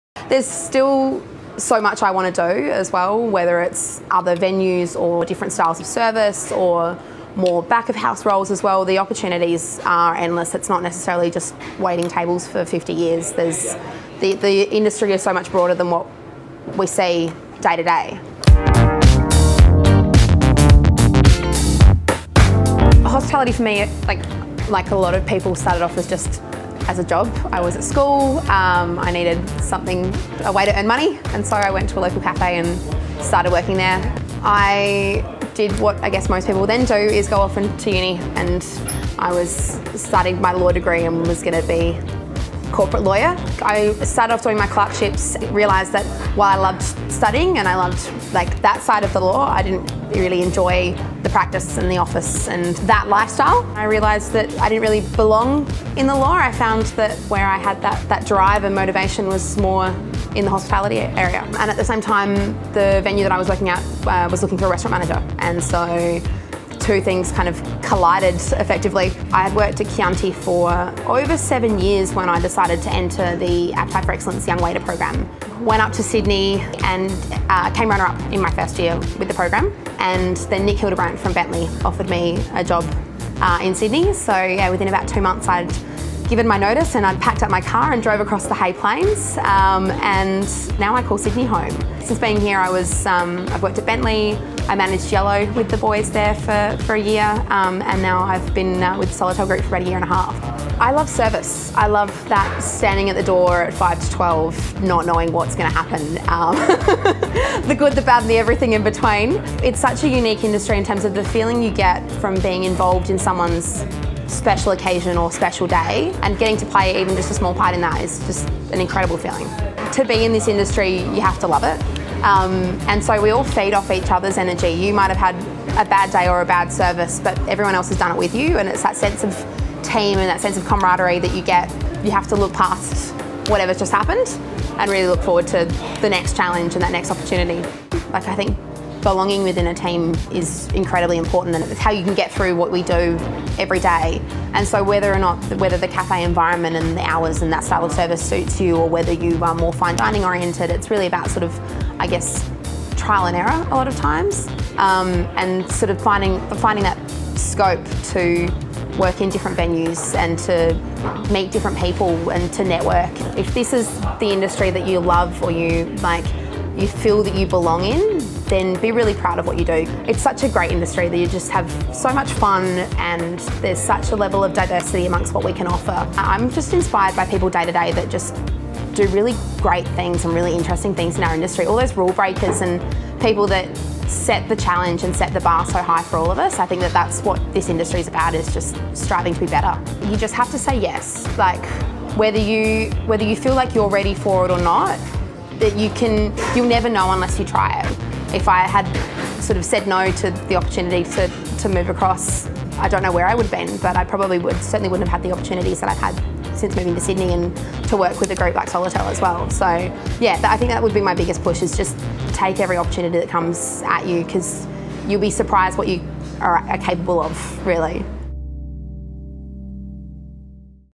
Humans Of Hospo: Brooke Adey
Brooke tells us about her story from law student, to packing her life up in Adelaide, and becoming the Restaurant Manager at Chiswick. Since filming, Brooke has become the Restaurant Manager of Bea at Barangaroo House.